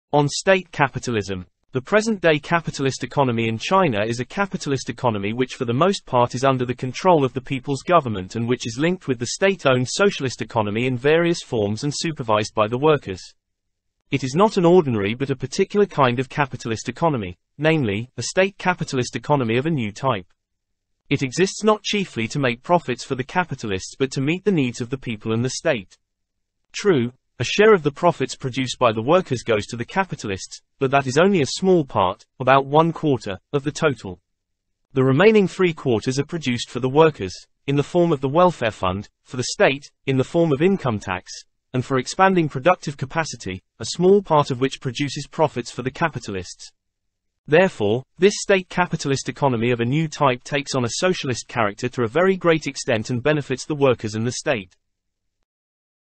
Mao - On State Capitalism
Mao - On State Capitalism [comment on a document of the National Conference on Financial and Economic Work held in the summer of 1953].

Text: The present-day capitalist economy in China is a capitalist economy which for the most part is under the control of the People's Government and which is linked with the state-owned socialist economy in various forms and supervised by the workers. It is not an ordinary but a particular kind of capitalist economy, namely, a state-capitalist economy of a new type. It exists not chiefly to make profits for the capitalists but to meet the needs of the people and the state. True, a share of the profits produced by the workers goes to the capitalists, but that is only a small part, about one quarter, of the total. The remaining three quarters are produced for the workers (in the form of the welfare fund), for the state (in the form of income tax) and for expanding productive capacity (a small part of which produces profits for the capitalists). Therefore, this state-capitalist economy of a new type takes on a socialist character to a very great extent and benefits the workers and the state.